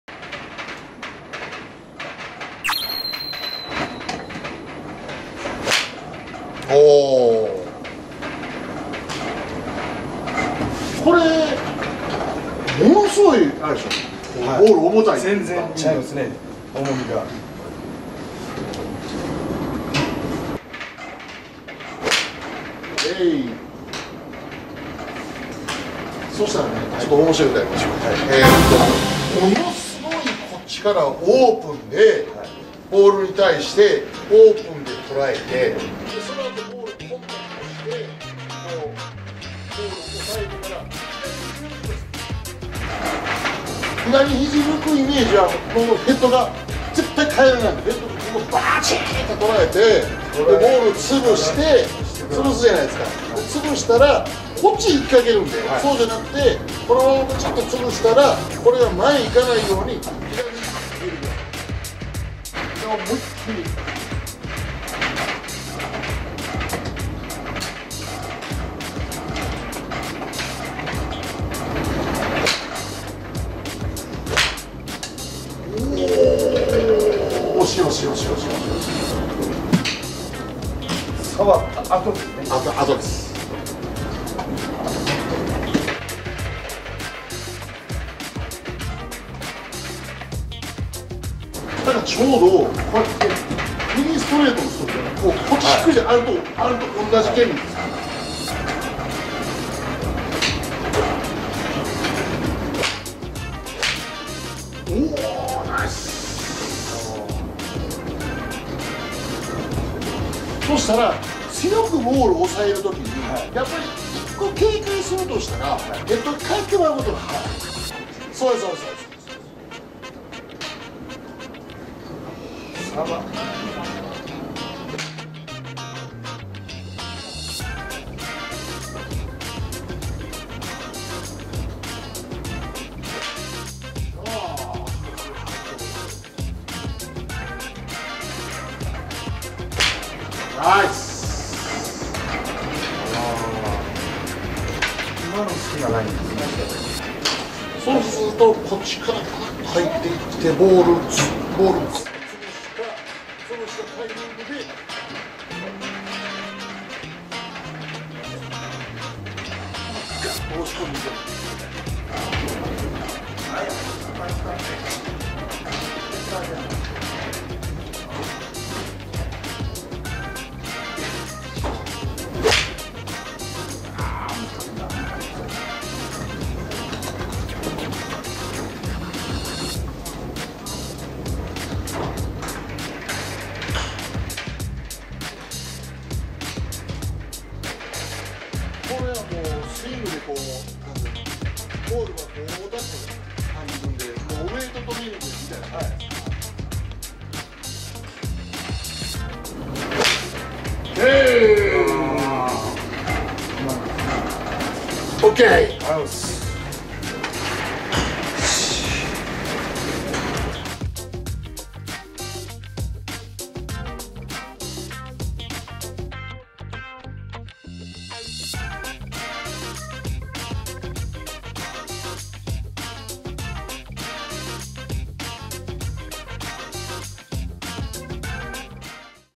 山本道場ゴルフTV📺 アマチュア強化企画👍〜強いボールを打つために✋〜
山本道場ゴルフTV📺
アマチュア強化企画がスタートです👍

山本道場にはジュニアだけではなく、たくさんのアマチュアゴルファーも所属しています。
そのアマチュアの方々をネイティブ・スイング理論にて強化していく様子をご覧ください✋

今回はボールの強化！！
スイングのウェイトトレーニング👍
です。